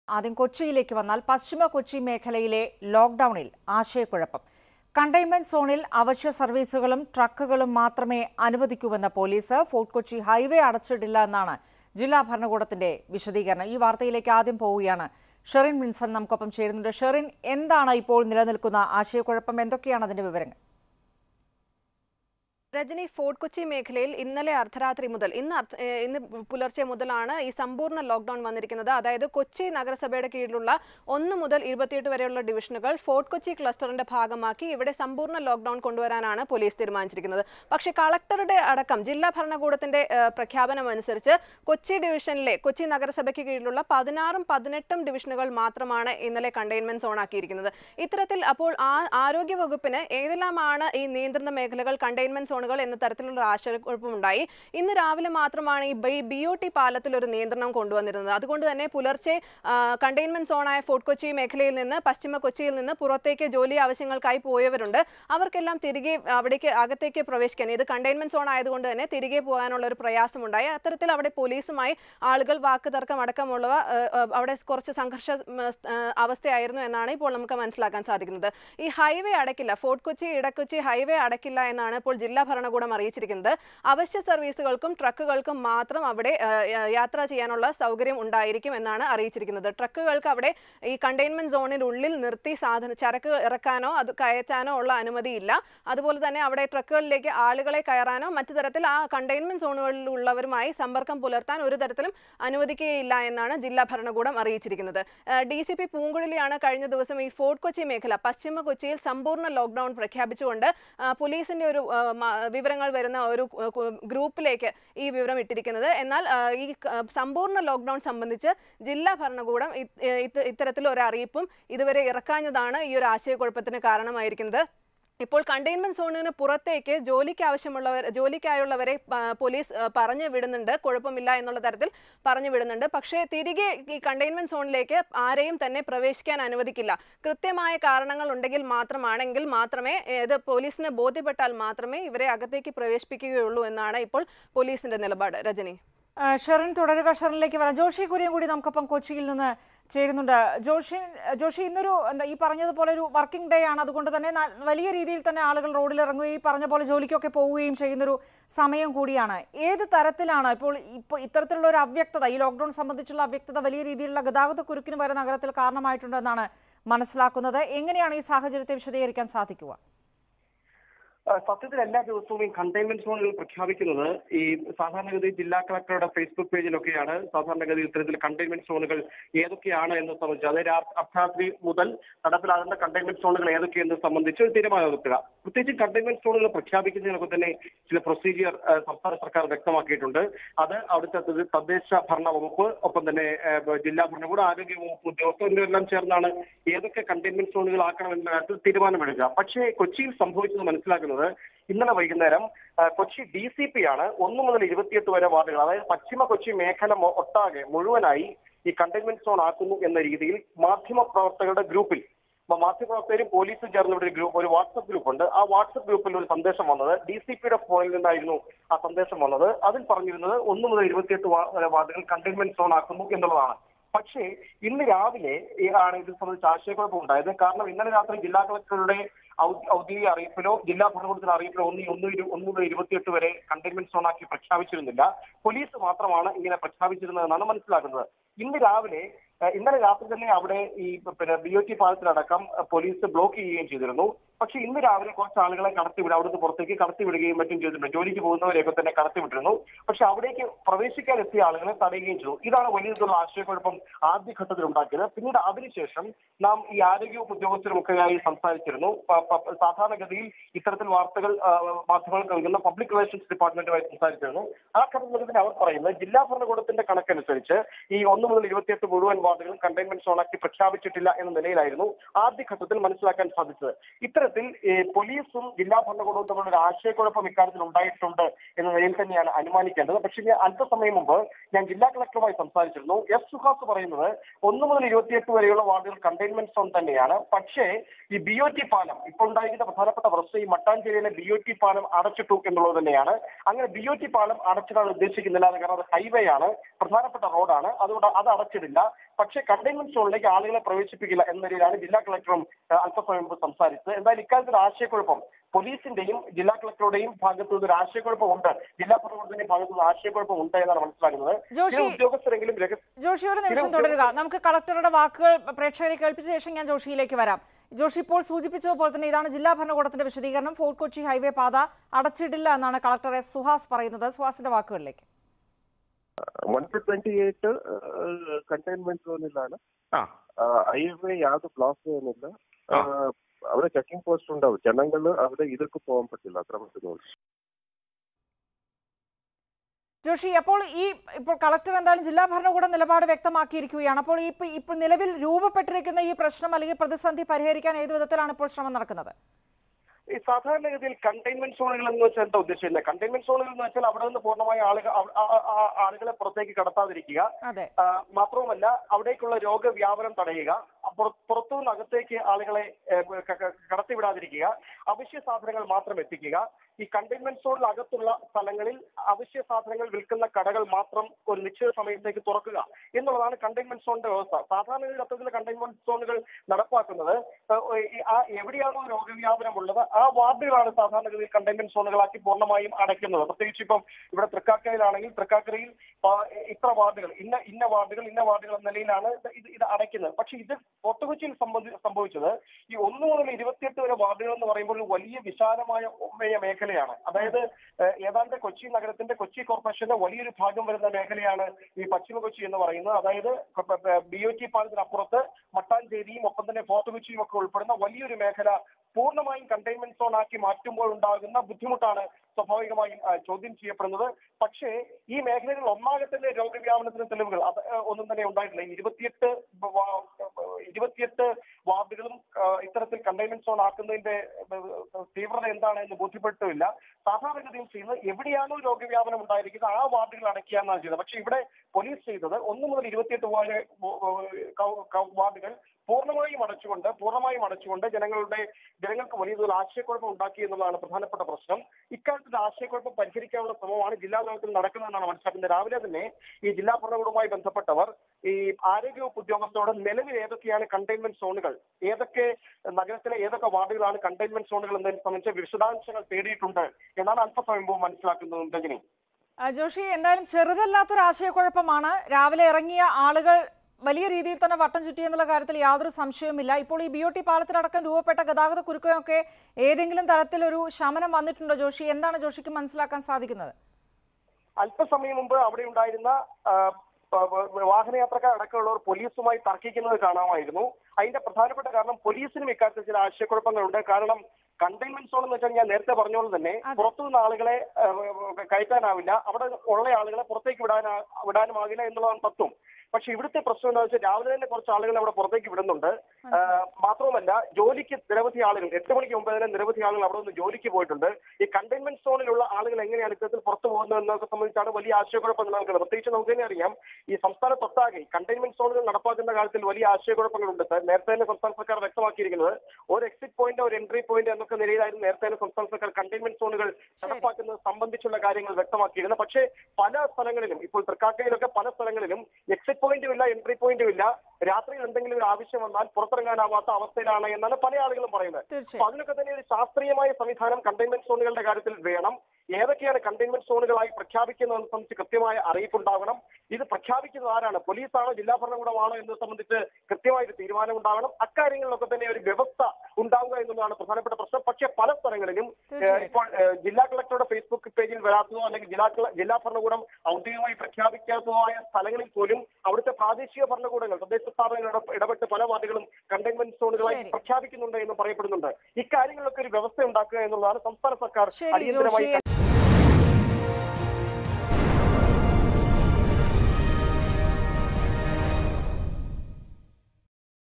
പശ്ചിമ കൊച്ചി മേഖലയിൽ ലോക്ക്ഡൗണിൽ ആശയകുഴപ്പം, ഫോർട്ട് കൊച്ചി ഹൈവേ അടച്ചിട്ടില്ലെന്ന് ജില്ലാഭരണകൂടം
fort kochi lockdown and containment zone confusion